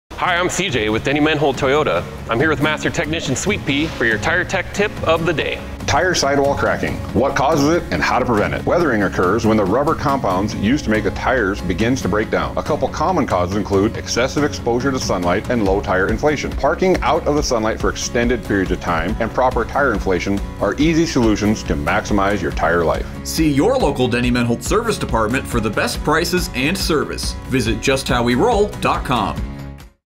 Tire Tech Tip - Avoid Tire Sidewall Cracking
Listen to our technician Sweet P and learn how to best avoid tire sidewall cracking to prolong your tire life.

Denny Menholt Toyota
1920 E Mall Dr

The right cars. The right prices. The right experience.